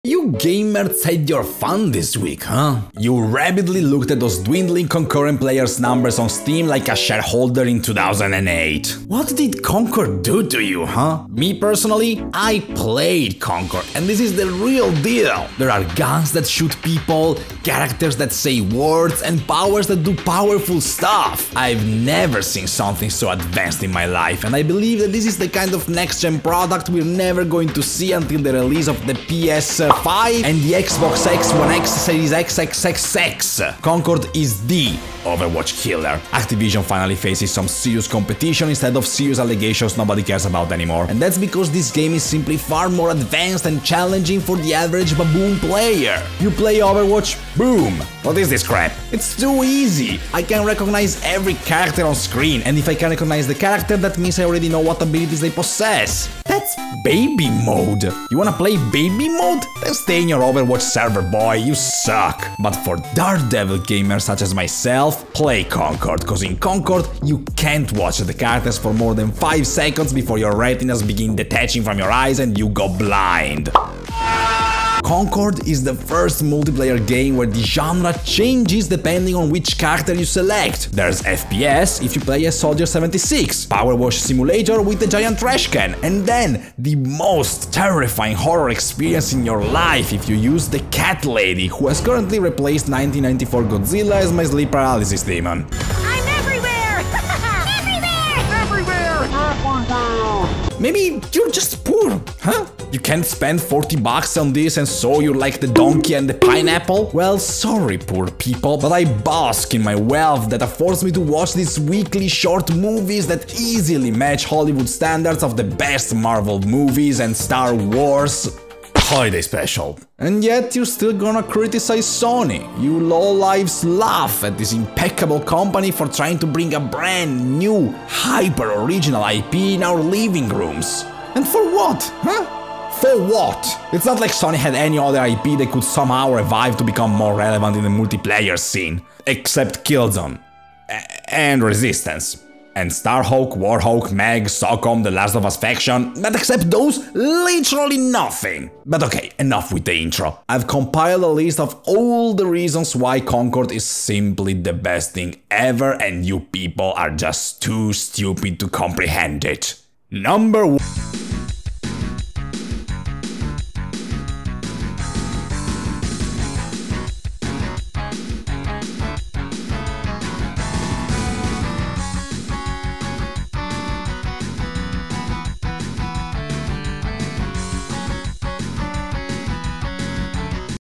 In Defense Of Concord
Concord is my new favorite multiplayer game. Tune in next week to listen to me explaining why Far Cry 6 is the best open world ever made